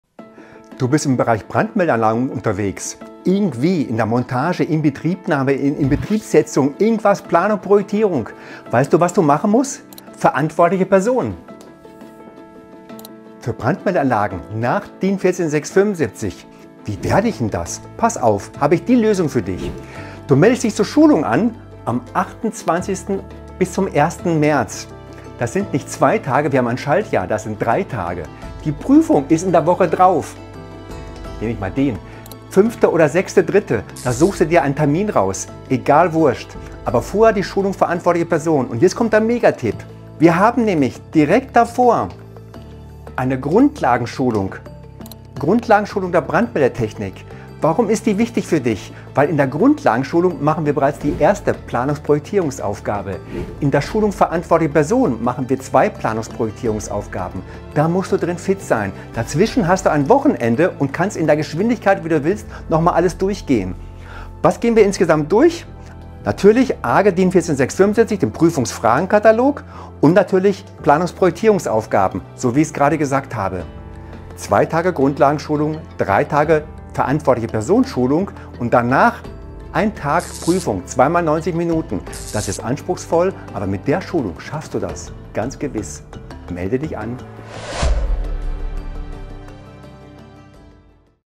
DIN 14675: Online-Schulung zur Verantwortlichen Person BMA
Bist du fasziniert von Brandmeldeanlagen 🔥 und strebst danach, die Verantwortliche Person BMA nach DIN 14675 im Unternehmen zu sein?

Dann sichere dir am besten noch heute deinen Platz in unserer bevorstehenden Online-Schulung:

📌 „Verantwortliche Person BMA nach DIN 14675“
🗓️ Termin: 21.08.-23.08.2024

Das Beste daran ist, dass die Prüfung zur Verantwortlichen Person für BMA, sobald bestanden, lebenslang gültig ist. Sie ist personenbezogen und nicht an das Unternehmen gebunden. Es wird keine Wiederholungsprüfung erforderlich sein!

Hier noch ein Pro-Tipp: Unser Webinar „Grundlagenschulung der Brandmeldetechnik nach DIN 14675“ eignet sich hervorragend als Vorbereitung auf die Schulung zur Prüfung "Verantwortliche Person BMA nach DIN 14675“.

Melde dich am besten zu beiden Schulungen und erhöhe die Wahrscheinlichkeit, die Prüfung zu bestehen.

Wir freuen uns darauf, gemeinsam mit dir die Welt der Brandmeldetechnik zu erkunden. Melde dich heute an und sichere dir deinen Platz!